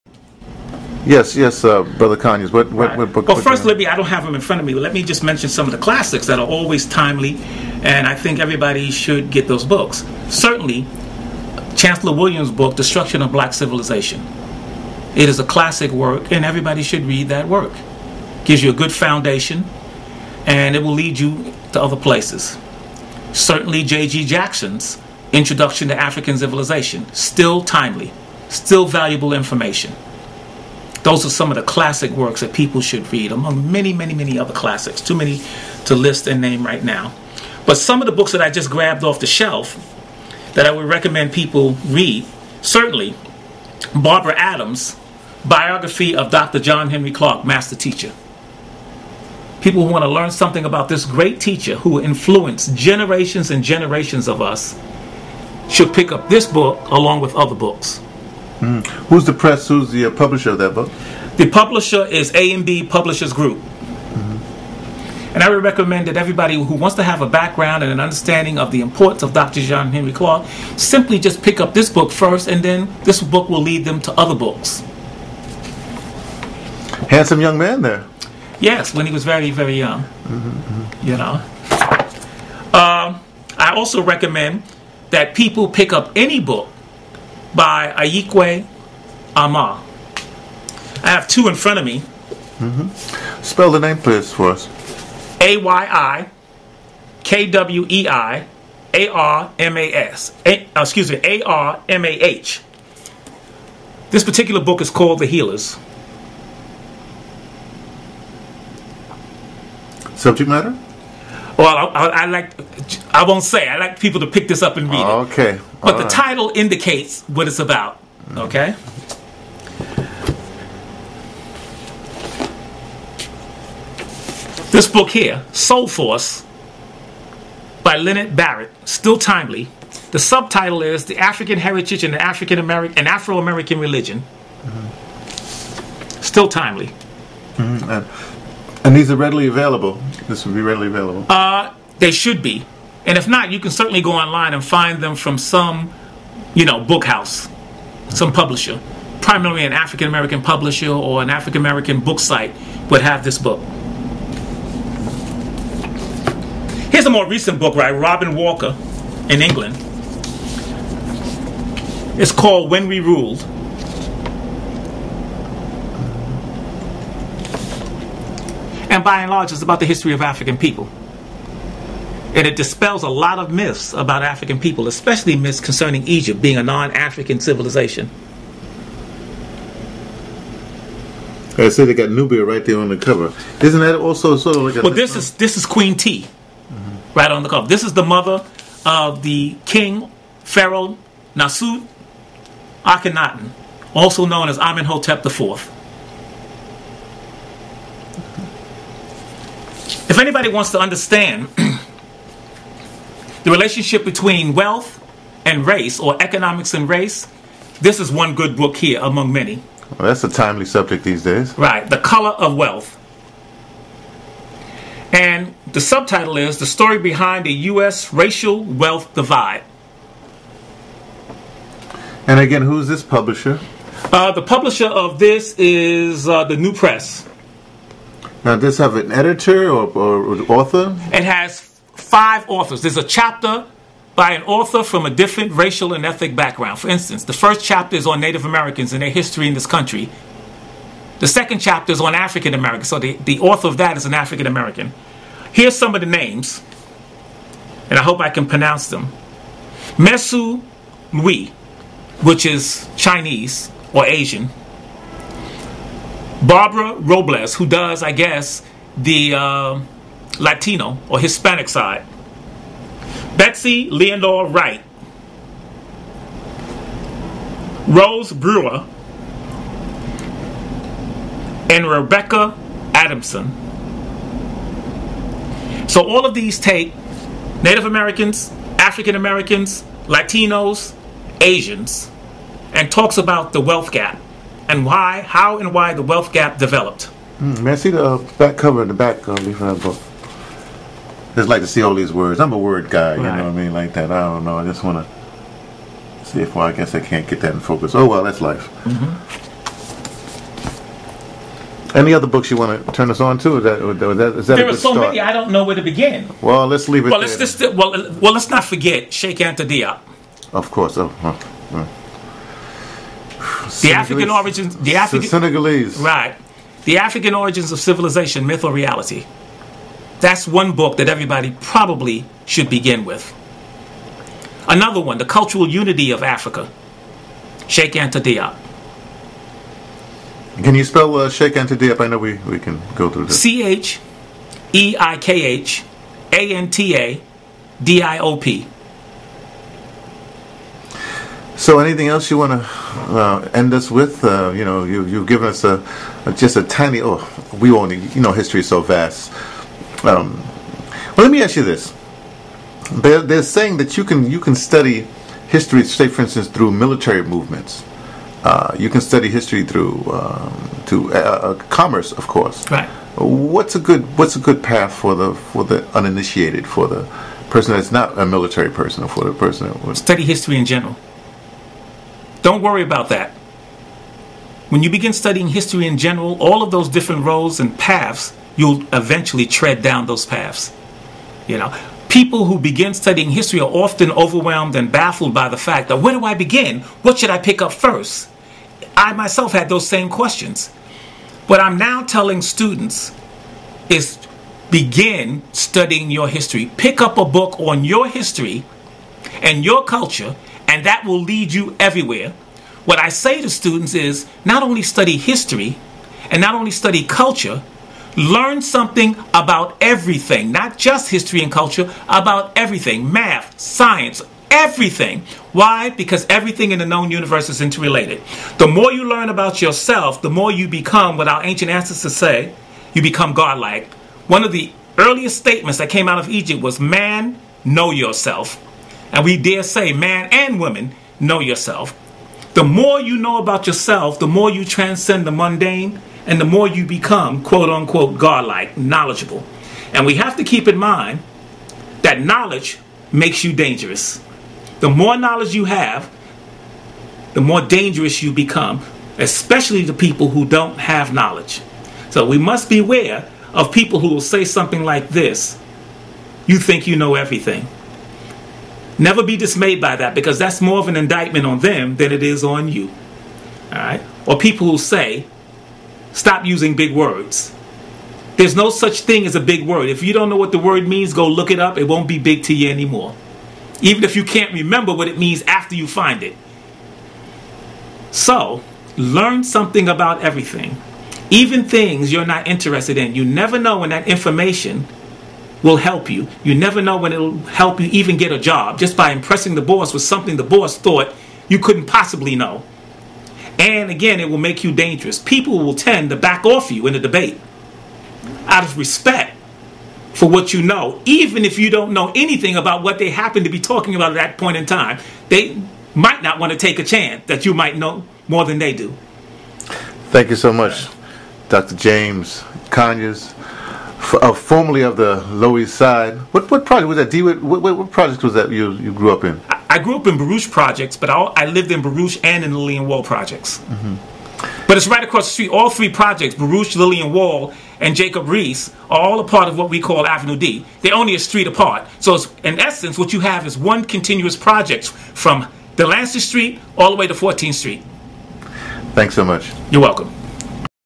Historian shows...and suggests basic Black History books
Professor James "Buddy" Conyers suggests African history books you should have access to for basic study in African American (and by extension) African history.

BITCOIN CONTRIBUTIONS:
722fce22-5e9f-42fc-a378-2da90f5d267a